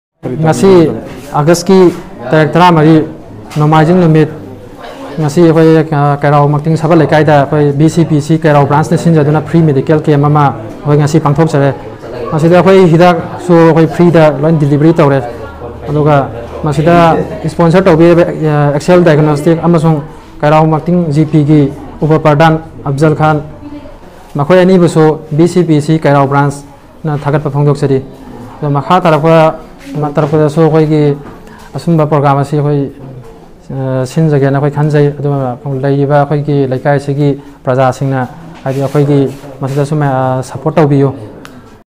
Abdul Waris (General Secretary, VCPC-MANIPUR Keirao Branch)
Abdul Waris (General Secretary, VCPC-MANIPUR Keirao Branch) na Sinkhiba  Free Medical Camp ama pangthokhiba